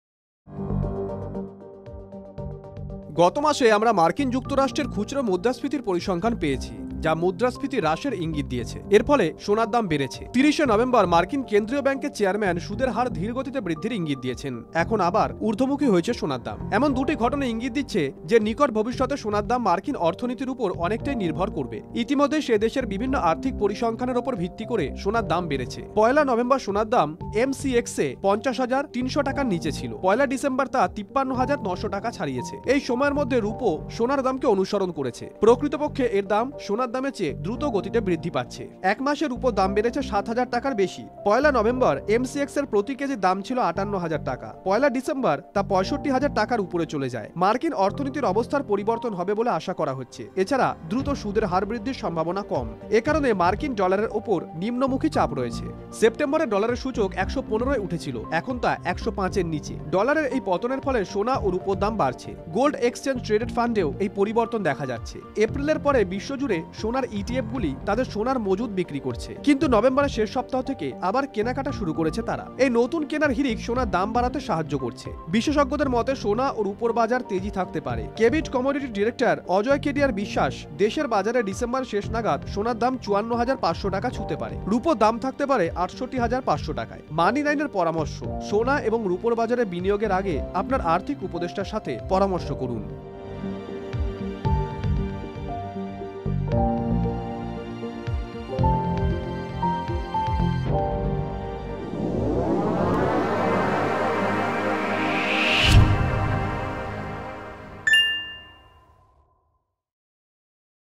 Gold| ডলার দুর্বল হচ্ছে, তেজি সোনা-রুপো| Dollar| Silver| Investment| Money9 Bangla| Repo rate
এই সময়ের মধ্যে রুপো সোনার দামকে অনুসরণ করেছে। প্রকৃতপক্ষে, এর দাম সোনার দামের চেয়ে দ্রুত গতিতে বৃদ্ধি পাচ্ছে।

এক মাসে রুপোর দাম বেড়েছে ৭ হাজার টাকার বেশি। ১লা নভেম্বর, MCX-এ প্রতি কেজির দাম ছিল ৫৮ হাজার টাকা। যা ১লা ডিসেম্বর তা ৬৫ হাজার টাকার উপরে চলে যায়।

মার্কিন অর্থনীতির অবস্থার পরিবর্তন হবে বলে আশা করা হচ্ছে। এছাড়াও, দ্রুত সুদের হার বৃদ্ধির সম্ভাবনা কম। এ কারণে মার্কিন ডলারের ওপর নিম্নমুখী চাপ রয়েছে।

সেপ্টেম্বরে ডলারের সূচক ১১৫-এর স্তরে, এখন তা ১০৫-এর নিচে রয়েছে। ডলারের এই পতনের ফলে সোনা ও রুপোর দাম বাড়ছে।